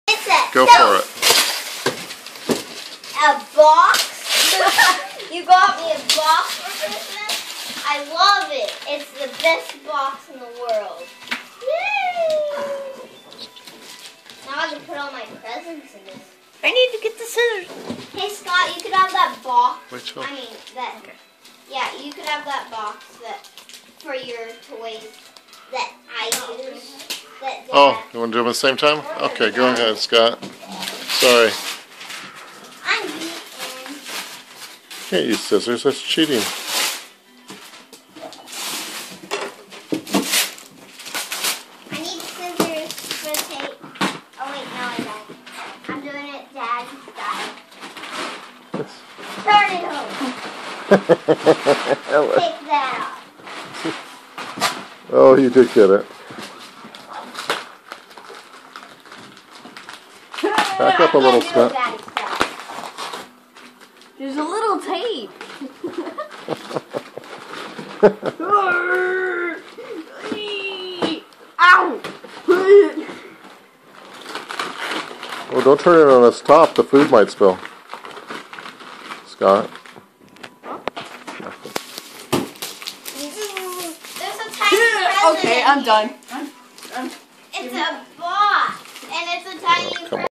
Christmas2012
Scott and Phoebe opening presents